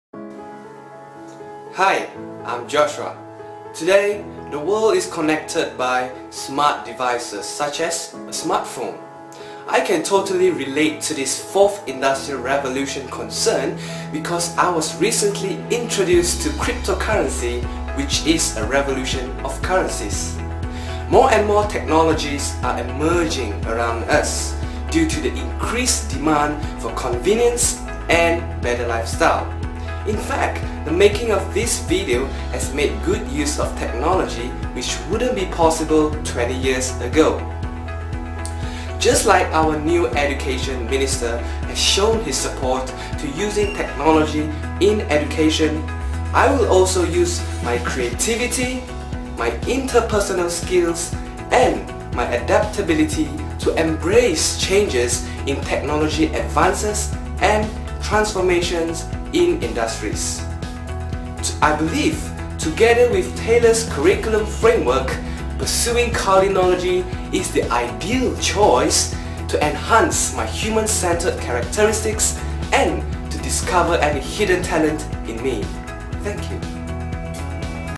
Taylor’s World Class Scholarship (TWCS) 2018 | Joshua Liang - Video Essay Application
Taylor’s World Class Scholarship (TWCS) 2018 Video Essay Application by Joshua Liang Qin Yew. "Why do you think the Fourth Industrial Revolution is inevitable and how would you prepare yourself for it?"